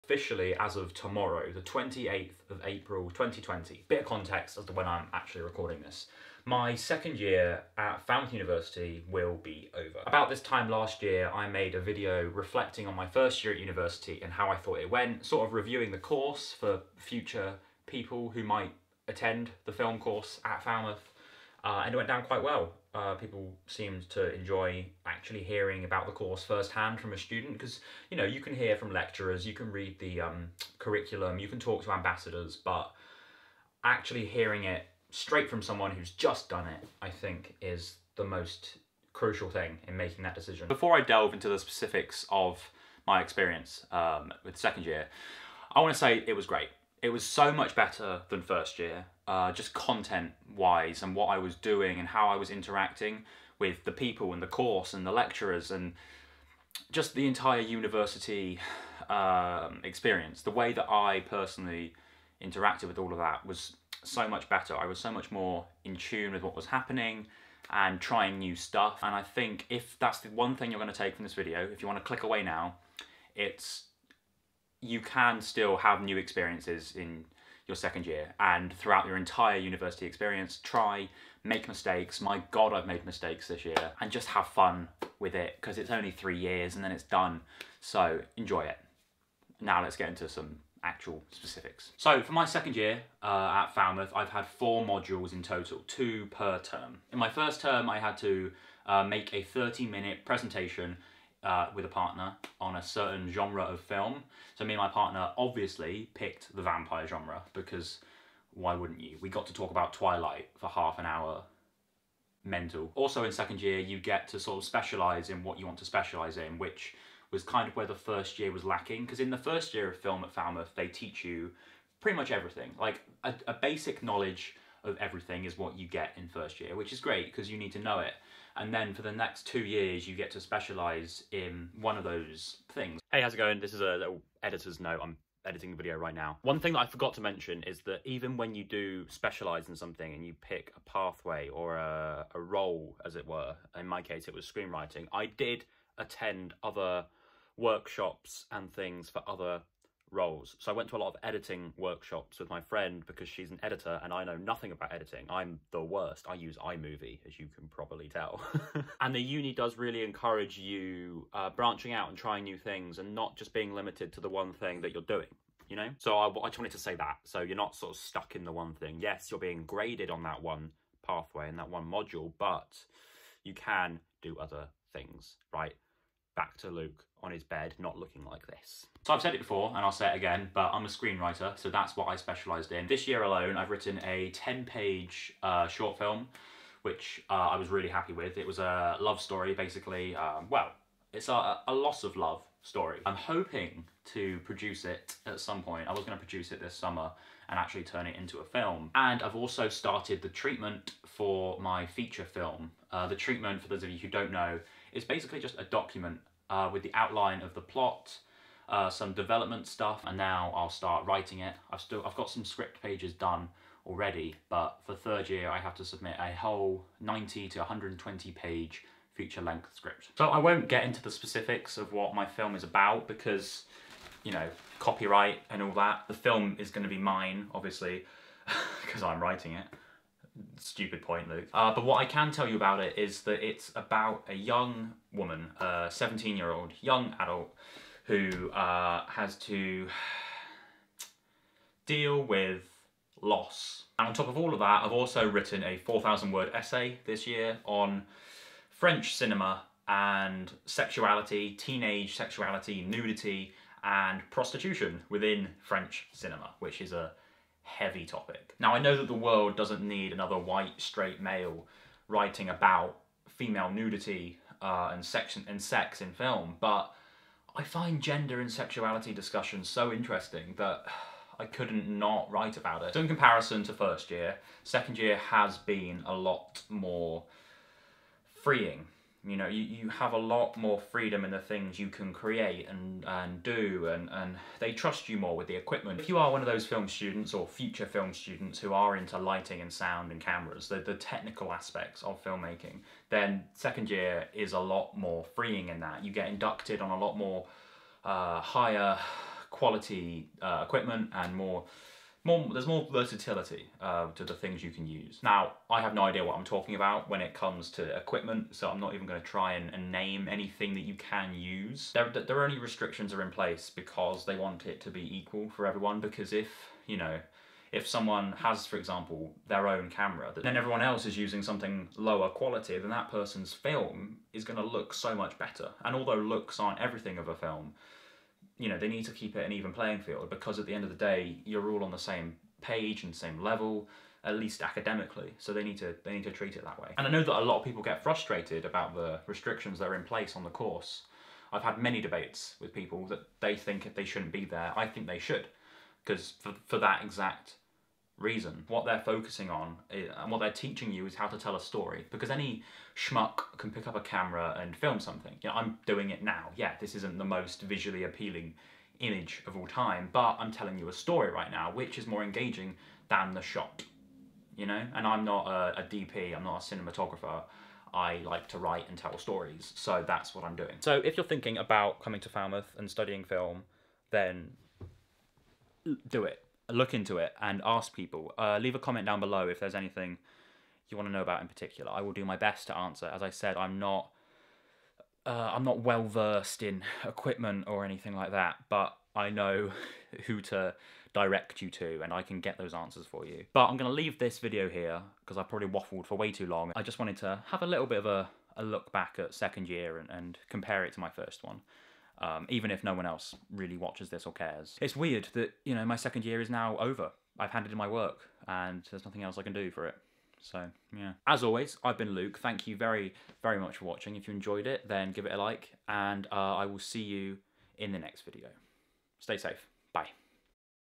Studying Film At Falmouth University - An Honest Review From A 2nd Year
As of April 28th, I've officially finished 2nd Year of Film at Falmouth University! I give the year a little review, feedback on the course and what I've done this year, with a hint as to what my Feature-length Screenplay will consist of!

In this video, I talk about the modules at Falmouth University, the campus experience, what you will study as a Film student, the equipment available to you and more. If you're interested in Falmouth University, studying film or being a screenwriter - this is the video for you!

Let me know if you have any more Falmouth Uni questions.